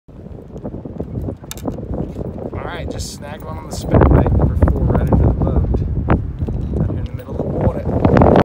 A bass on a spinner bait
3 of 4 fish I caught on the day at Lake Denmark 3/14/2019